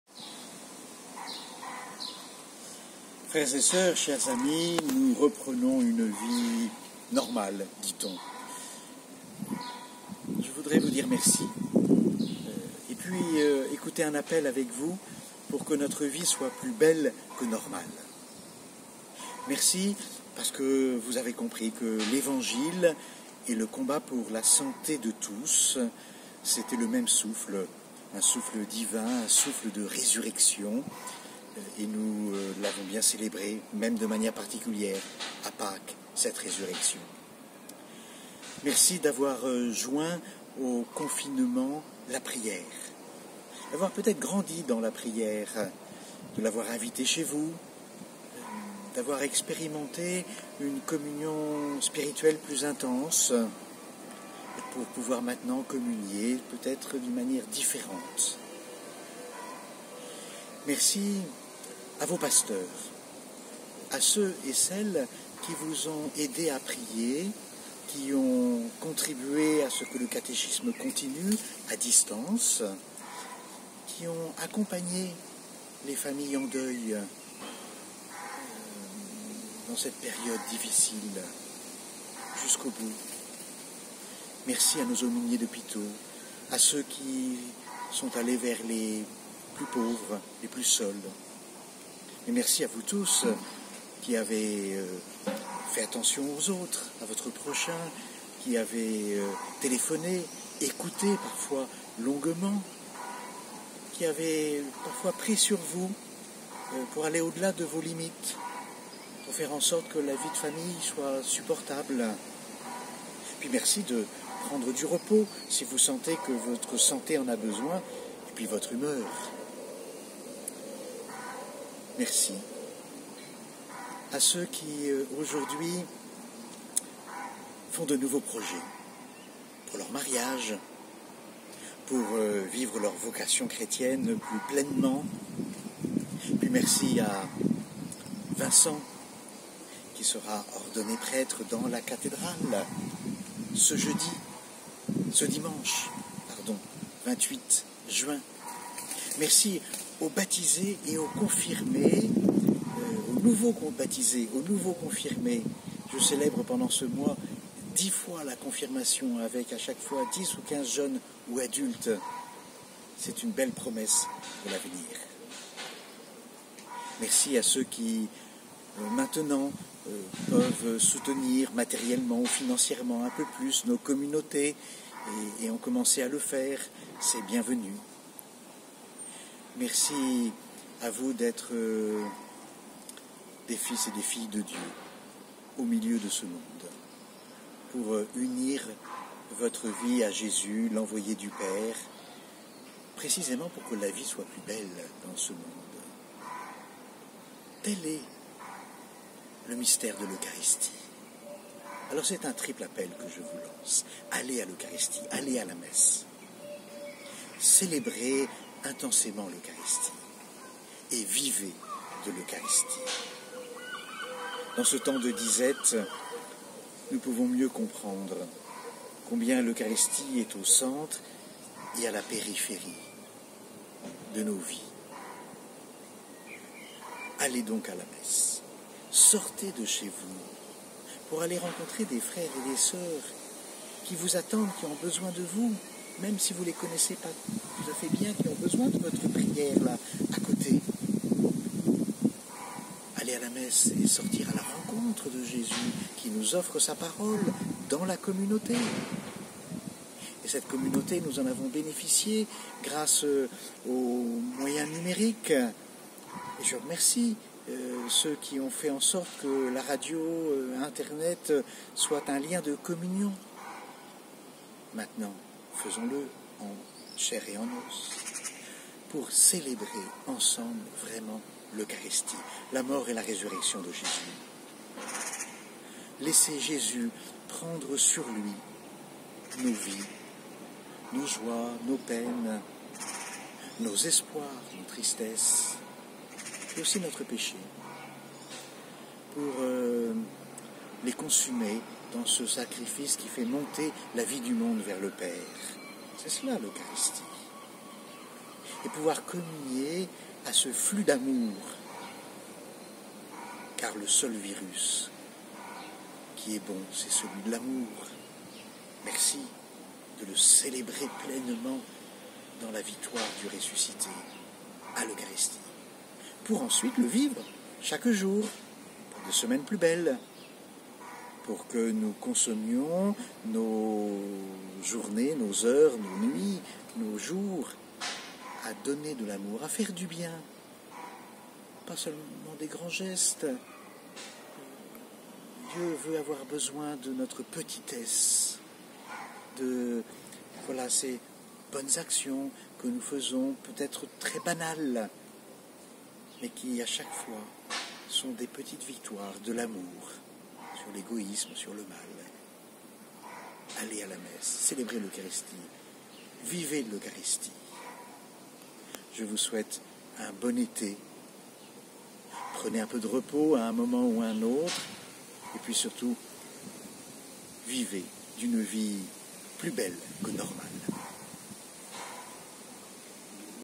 Message de Mgr Dominique Lebrun aux fidèles du diocèse de Rouen (juin 2020)
Chers amis,

Nous reprenons une vie plus normale, dit-on. Je voudrais vous dire « Merci »,
et entendre avec vous un appel pour une vie plus belle que normale.
Oui, merci d’avoir compris que l’Évangile et le combat pour la santé de tous sont du même souffle, le souffle de vie, le souffle de la résurrection.
Merci d’avoir joint au confinement la prière ; merci d’avoir grandi dans la prière en l’invitant chez vous, en la vivant seul ou à plusieurs, en communiant plus intensément et, pour certains, différemment.
Merci à vos pasteurs, aux personnes qui ont pris de nombreuses initiatives pour aider à la prière, au catéchisme à distance, à l’attention envers les plus seuls ou les plus pauvres, pour accompagner les familles en deuil ou en souffrance.
Merci d’avoir cherché à vivre l’épreuve avec Jésus et avec la communauté, d’avoir vu dans la radio ou l’ordinateur de bons médias pour nous relier les uns aux autres ; Merci à ceux qui ont permis ces liens devenus authentiquement spirituels.
Merci d’avoir fait attention à votre prochain, d’avoir téléphoné, d’avoir écouté parfois longuement ; Merci d’avoir pris sur vous pour que la vie de famille se passe au mieux, d’avoir supporté vos limites et celles de vos proches ;
Merci de vous reposer si vous sentez que la période est éprouvante pour votre santé ou votre humeur.
Oui, Merci d’avoir maintenu l’espérance et pas seulement l’espoir du déconfinement, d’avoir mis votre confiance en Dieu et dans son projet d’une humanité où la contagion de l’amour est le seul bon virus.
Merci à ceux qui peuvent soutenir la communauté en donnant un secours matériel ou financier bienvenu maintenant ;
Merci aux nouveaux baptisés et confirmés, à ceux qui font de nouveaux projets pour se marier ou pour vivre pleinement leur foi dans notre monde.

Merci à VINCENT ordonné prêtre ce 28 juin à la cathédrale !

Oui, merci d’être vraiment les fils et les filles de Dieu au milieu de notre monde,
sans prétention, avec la joie d’offrir notre vie en un sacrifice de louange et d’action de grâces en union avec celui de Jésus, dans la joie de l’Évangile.
Tel est l’enjeu de l’Eucharistie et le triple appel que je voudrais entendre avec vous : aller à l’Eucharistie, célébrer l’Eucharistie, vivre l’Eucharistie … Après ce temps de disette eucharistique, comprenons-nous mieux combien elle est au centre de notre vie de disciples de Jésus ?
Aller à la messe, c’est sortir de chez moi … aller vers des frères et des sœurs qui ont besoin de ma présence. Aller à la messe, c’est aller à la rencontre de Jésus qui me tend la main, qui me parle, mieux, qui me tend son Corps, livré pour moi et pour tous.
Célébrer l’Eucharistie, c’est, après avoir écouté la Parole, offrir ma vie en union avec Jésus, laisser Jésus prendre sur lui mes joies et mes peines, mes tristesses et mes espoirs, mon péché aussi, et accueillir son Corps victorieux, ressuscité, pour une nouvelle et belle semaine avec son Esprit Saint.
Vivre l’Eucharistie, c’est « consumer » mes jours et mes nuits dans la joie de me donner, de donner un peu d’amour, de vivre pour les autres, en passant par la case « pardon » autant de fois que nécessaire !
Chers amis, confiez vos mercis et vos désirs d’une vie plus belle à la Vierge Marie, Mère de l’Église. Que son amour maternel soit contagieux dans vos communautés et celles que vous rencontrerez pendant l’été !
Bon été, avec un bon repos à un moment ou un autre, et que votre vie soit plus belle que normale !

Mgr Dominique Lebrun
Archevêque de Rouen